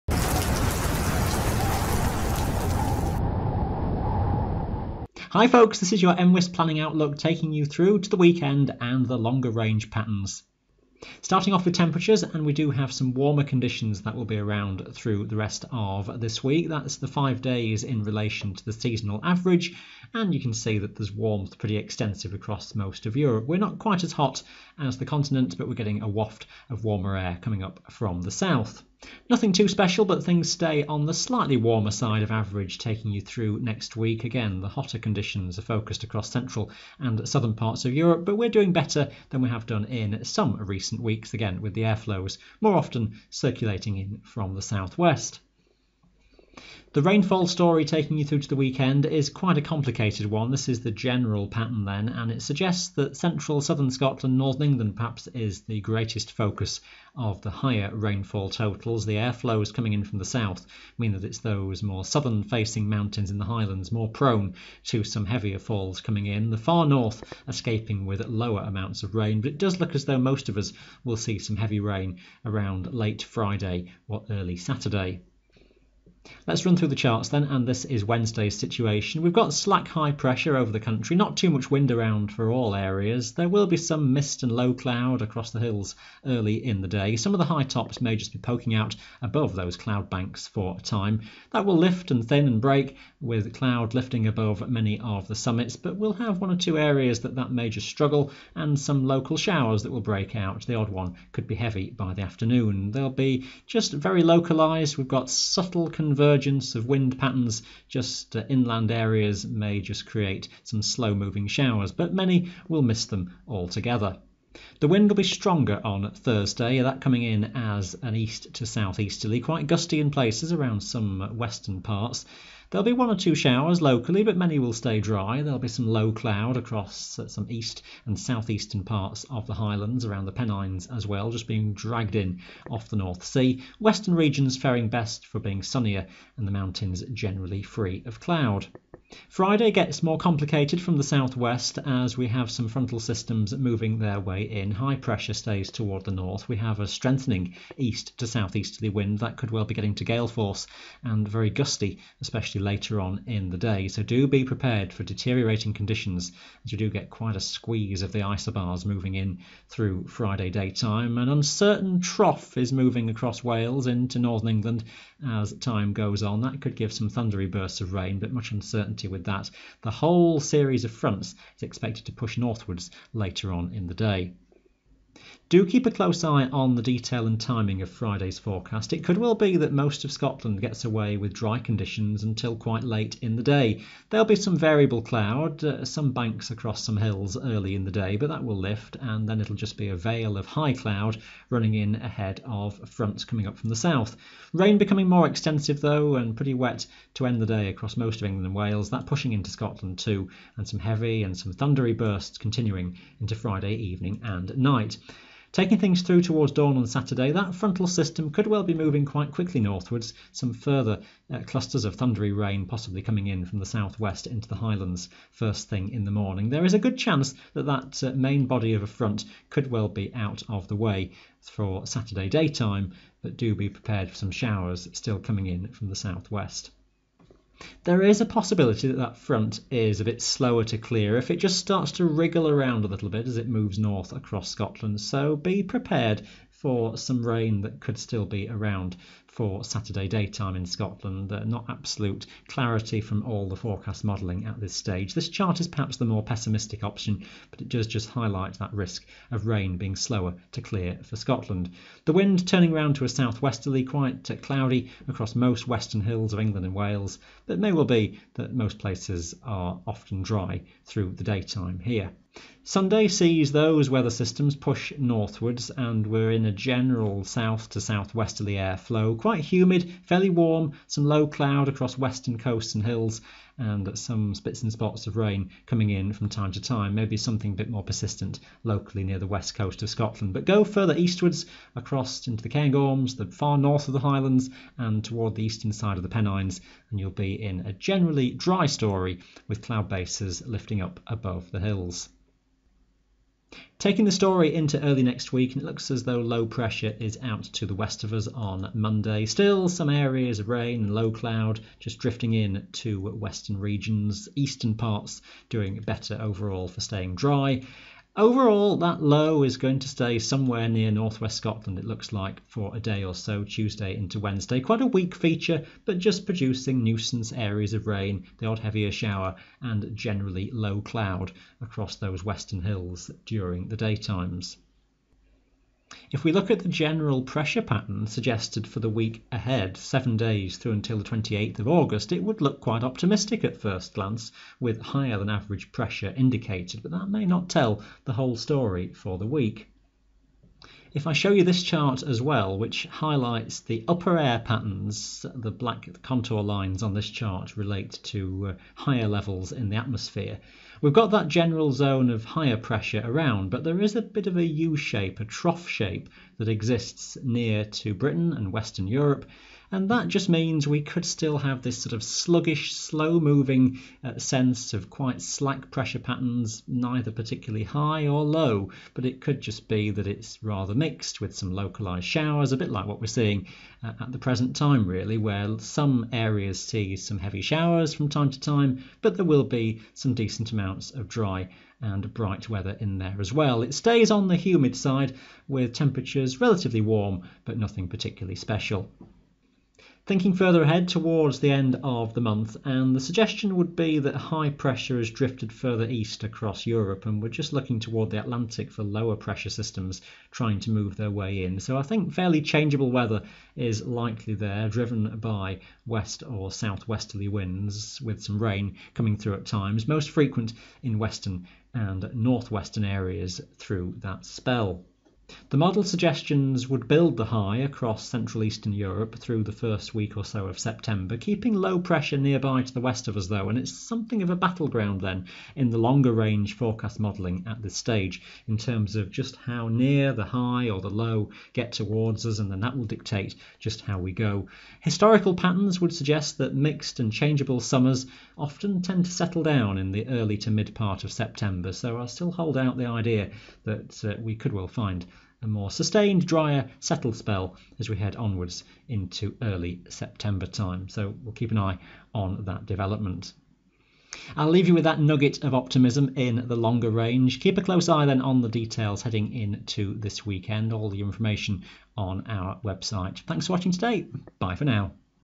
Planning Outlook (15/8/23)
Our latest longer range discussion into the weekend and beyond

Settled or unsettled? It's a complicated story from the forecast modelling in the next few weeks.

3-day forecasts for 10 mountain areas of Britain updated daily